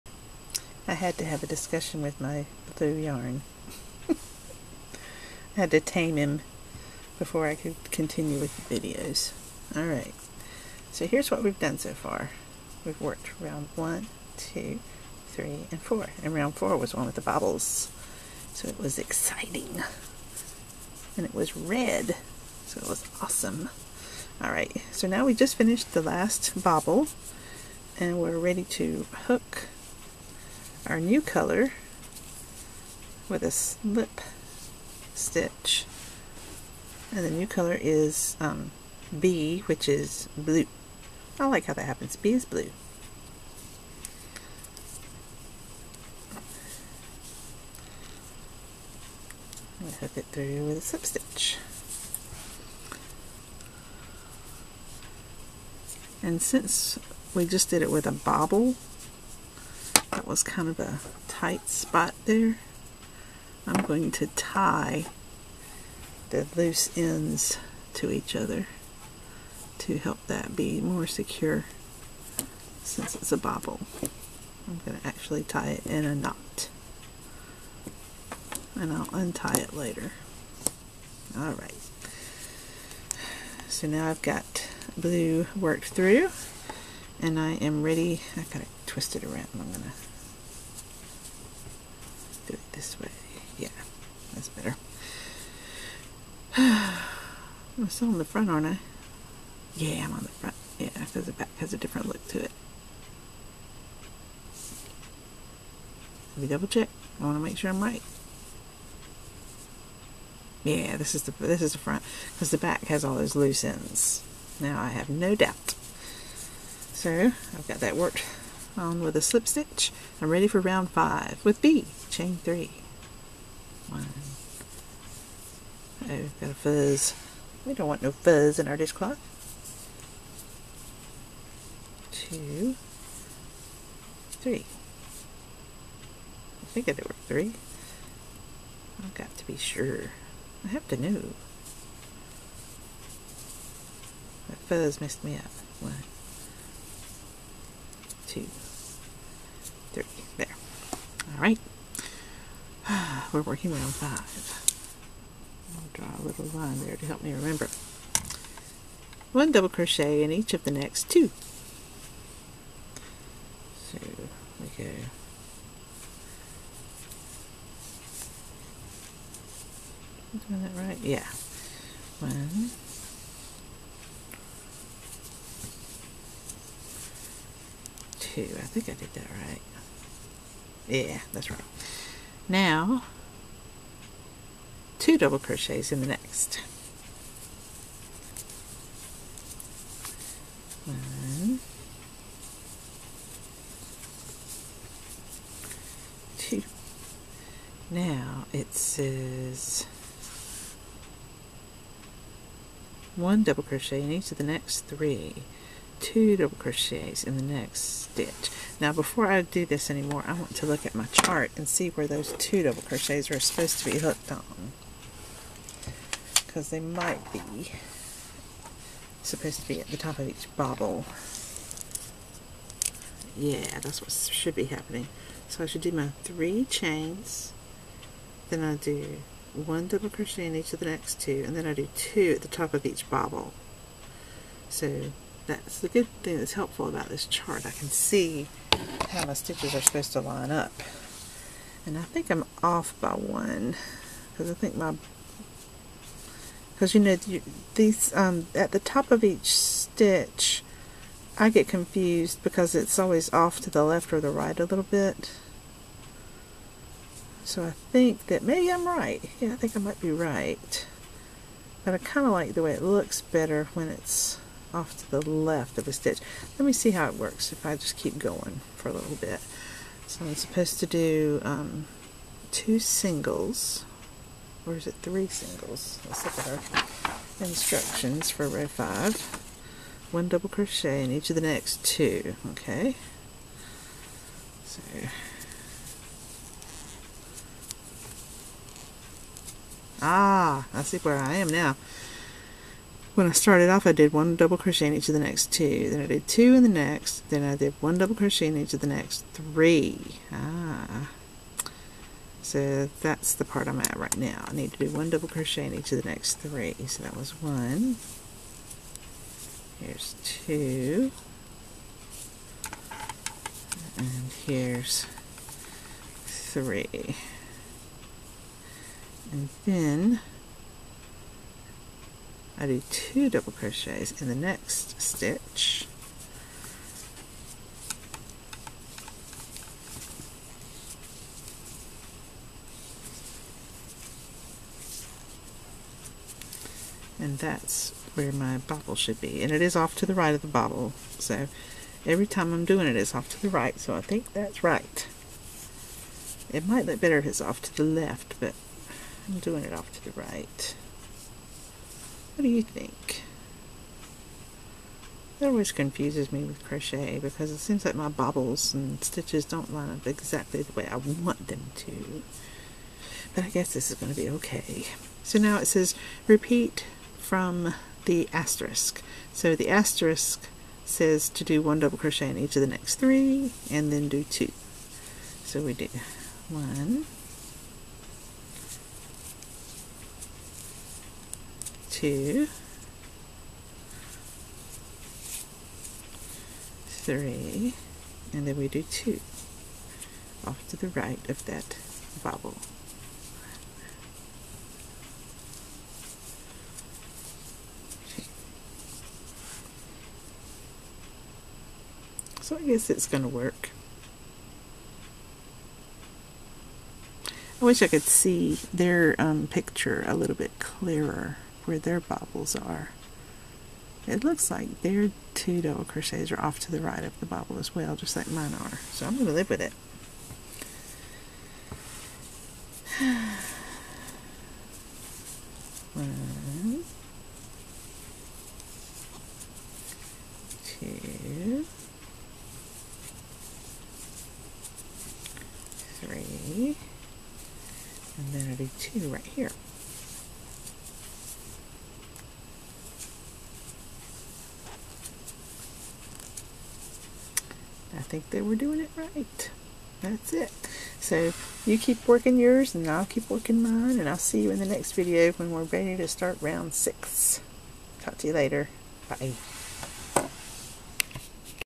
Valentine Dishcloth CAL #4 - Round 5
We are making this "Be My Valentine" dishcloth as a Crochet-Along in the Yarnspirations Forum. Come join us!
In this video, I work Round #5.